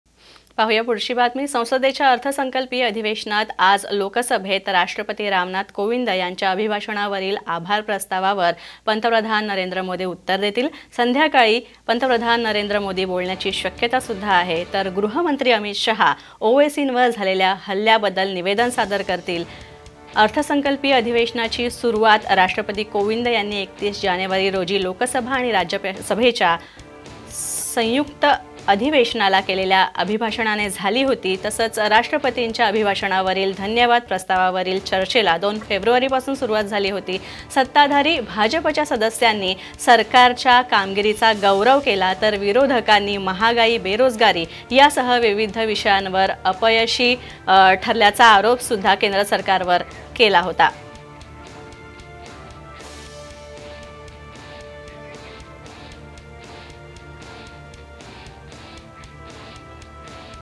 Narendra Modi : संध्याकाळी मोदींचे भाषण होणार, मोदी आभार प्रस्तावाला देणार उत्तर
Watch Latest News Updates on Narendra Modi : संध्याकाळी मोदींचे भाषण होणार, मोदी आभार प्रस्तावाला देणार उत्तर

Narendra Modi: Modi's speech will be held in the evening, Modi will respond to the motion of thanks